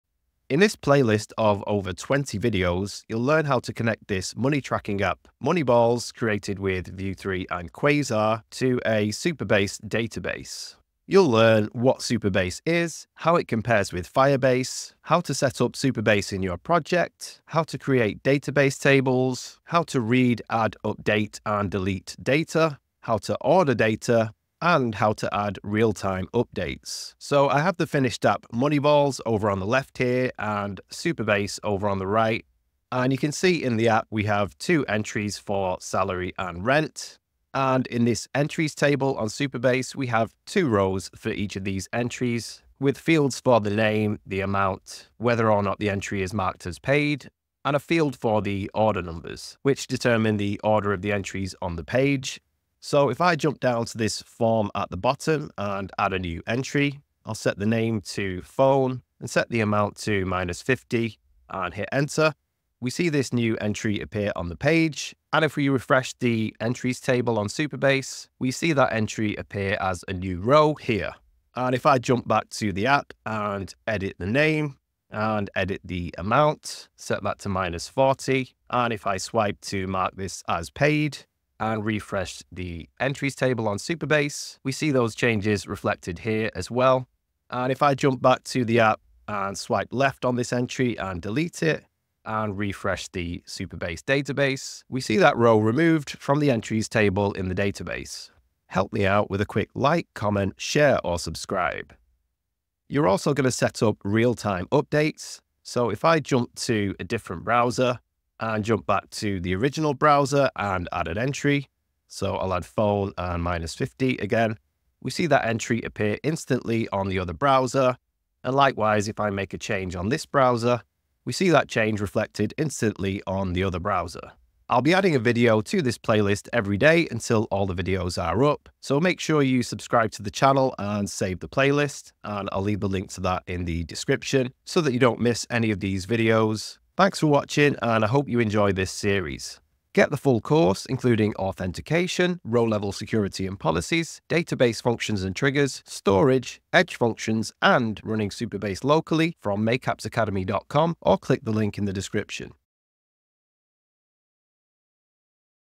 Supabase & Vue 3 Tutorial #1 - Introduction
Learn Supabase & Vue 3! #1 of 23 - Introduction

In this video I'll show you what you will learn in this series.